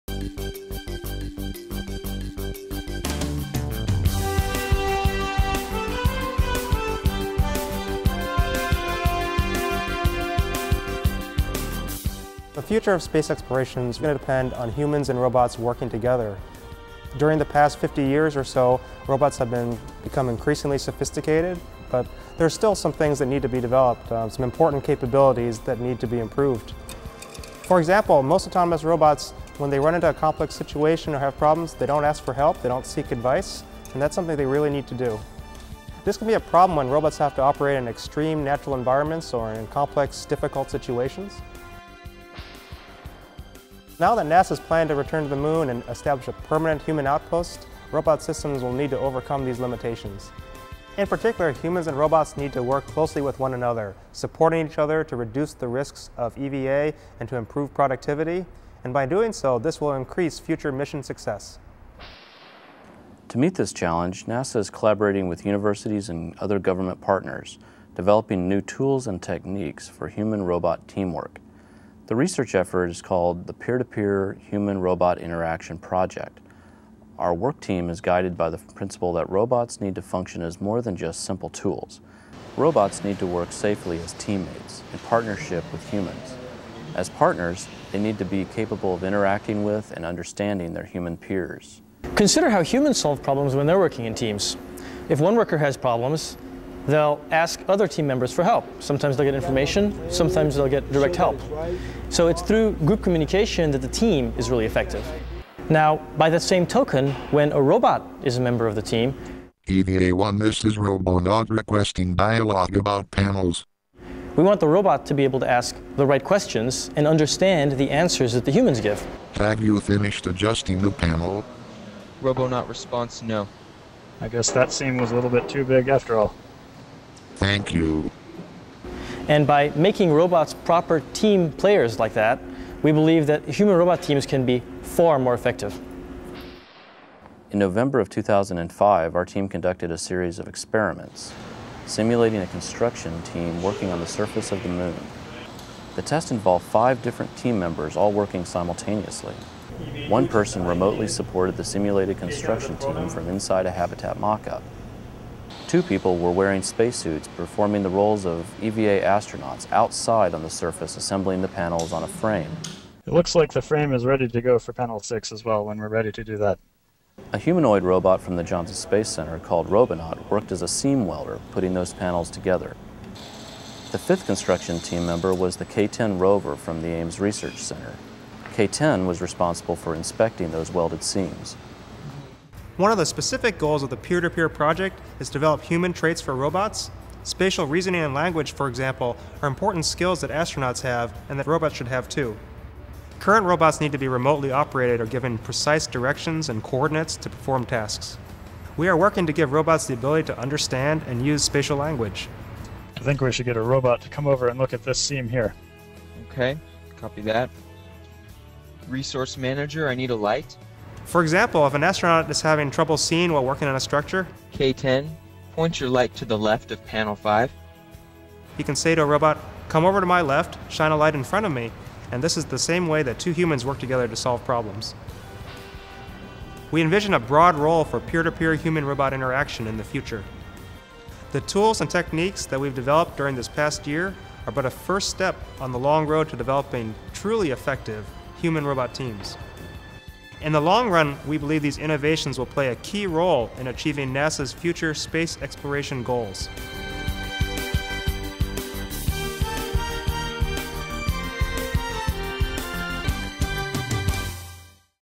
NASA P2P Human-Robot Interaction
The Peer-to-peer Human-Robotic Interaction Program will help NASA define the robots improvements needed for the "Return to the Moon" program.